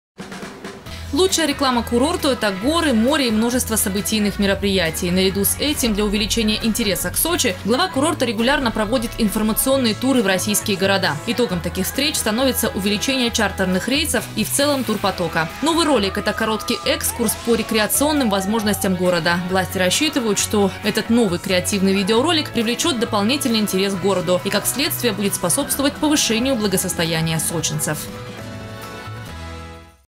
Новый промо-ролик о Сочи появился в ротации на федеральных каналах. Новости Эфкате
Его появлению способствовала системная деятельность администрации по продвижению Сочи на рынке туристских услуг.

Лучшая реклама курорту - это горы, море и множество событийных мероприятий. Наряду с этим для увеличения интереса к Сочи глава курорта регулярно проводит информационные туры в российские города. Итогом таких встреч становится увеличение чартерных рейсов и в целом турпотока.

Новый ролик - это короткий экскурс по рекреационным возможностям города. Власти рассчитывают, что этот новый креативный видеоролик привлечет дополнительный интерес к городу. И, как следствие, будет способствовать повышению благосостояния сочинцев.

Сочи. Новости.
Телеканал Эфкате.